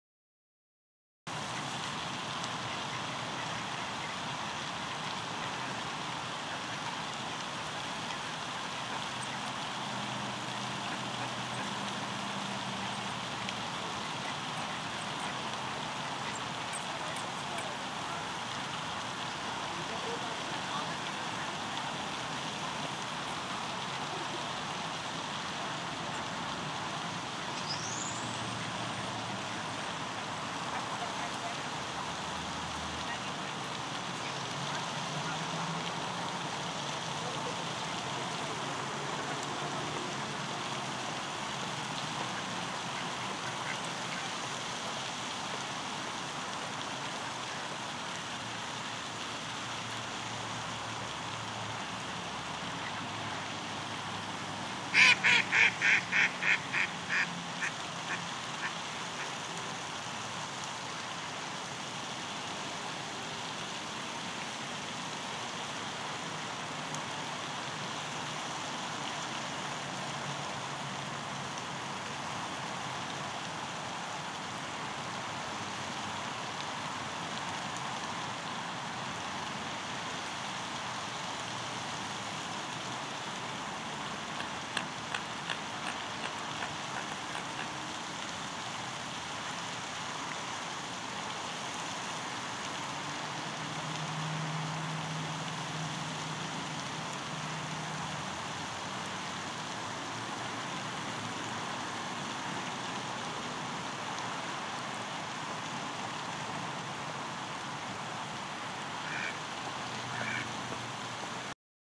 Great egret preening
A great egret preens itself at the Hutton Center in Santa Ana, California